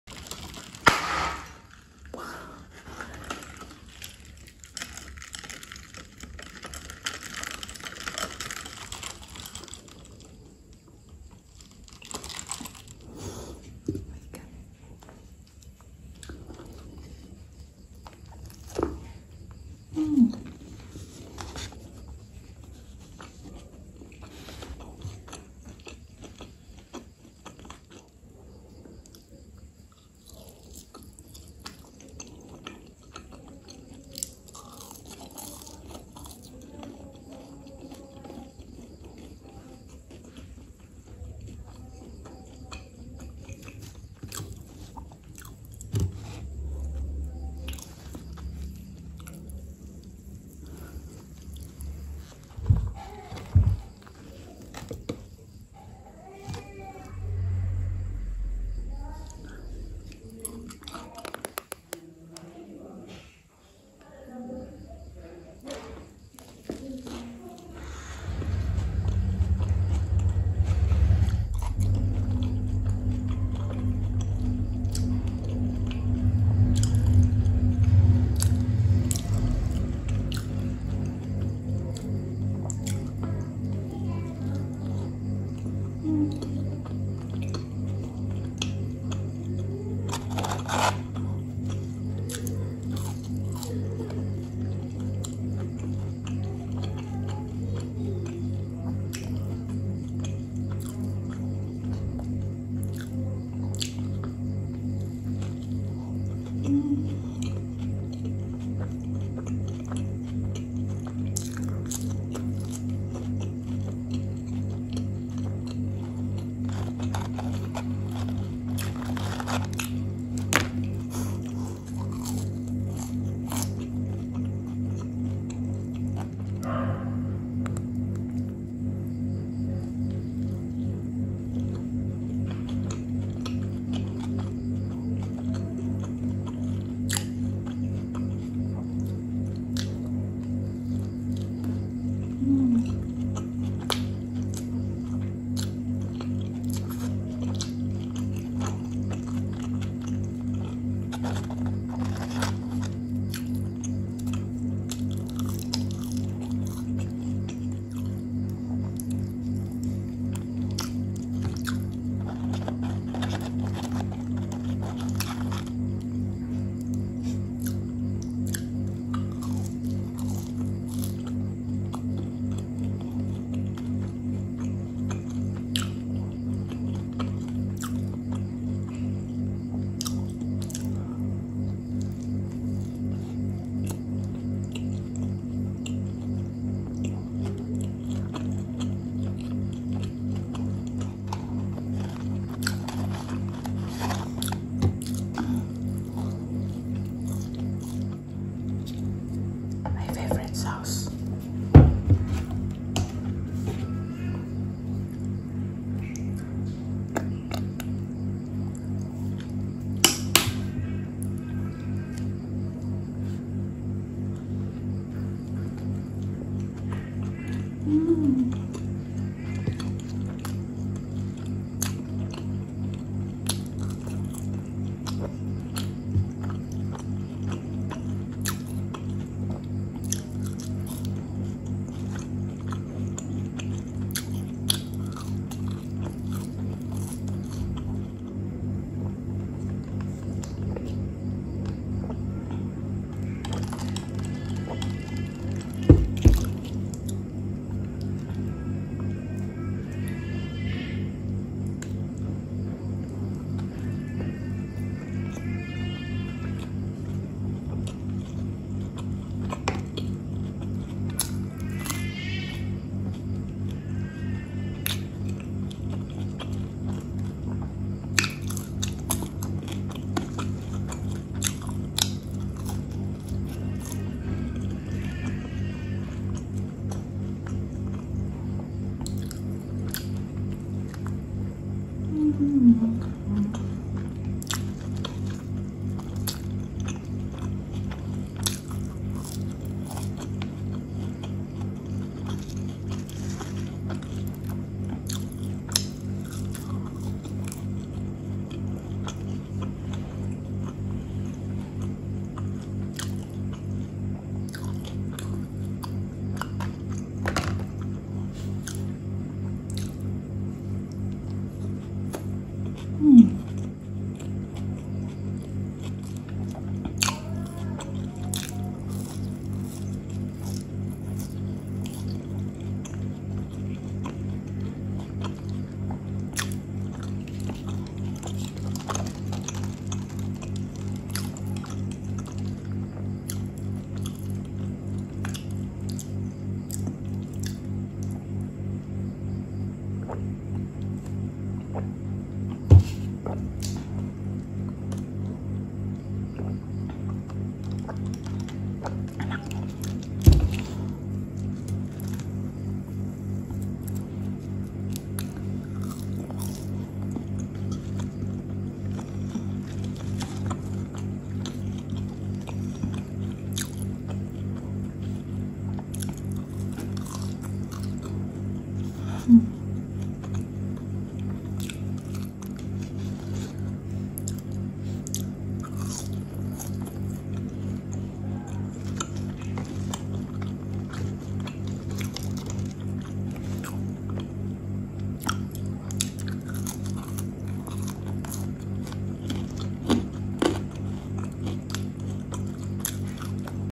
ASMR MUKBANG | MCDONALD'S (BIG MAC BURGER, FRENCH FRIES & GINGER ALE SODA) | ASMR INDONESIA
Burger favorite kalian apa di mcd? Kalo aku suka banget sama big mac! Tapi susah makannya karena gede banget! Ini aja aku sampe potong karena diawal video pas suapan pertama rahang aku sampe bunyi wkwk

What's your favorite burger on mcd? Mine is big mac for sure!